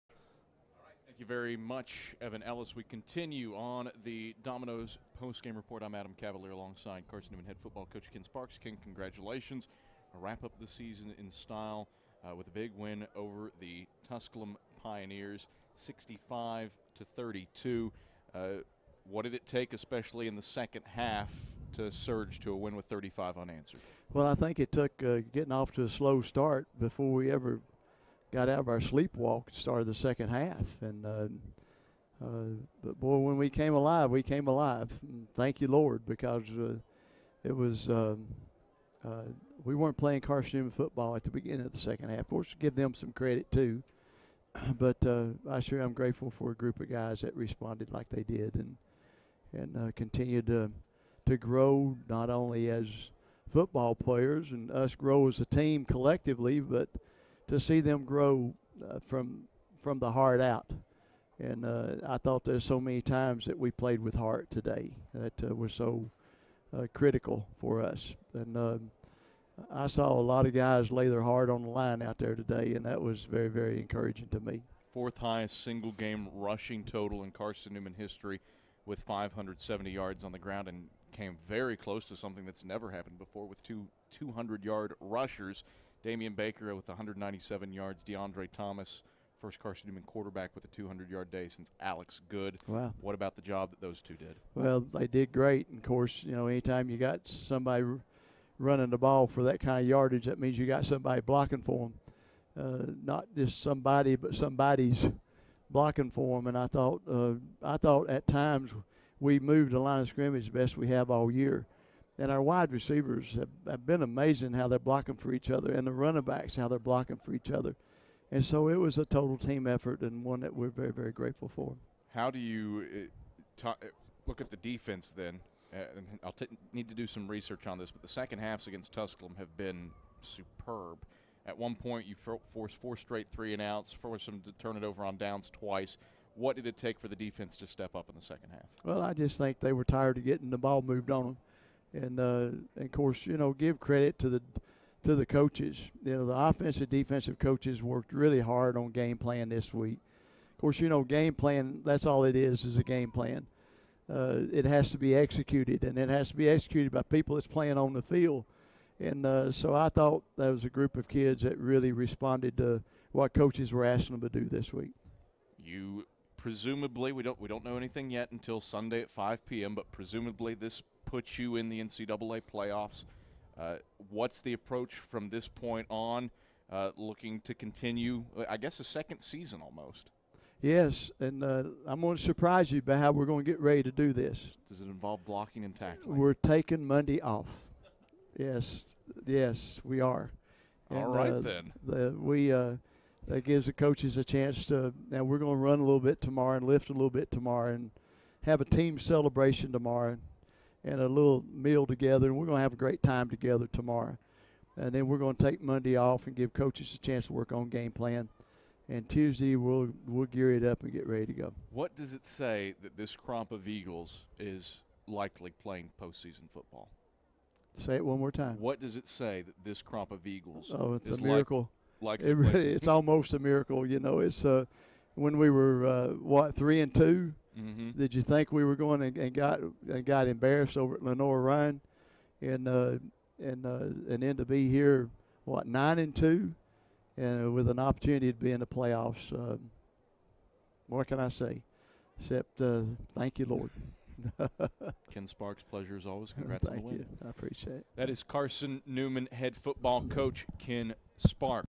Carson-Newman Football: Ken Sparks Post-TC 11-14-15
Eagles coach Ken Sparks breaks down a 65-32 win over the Pioneers on senior day.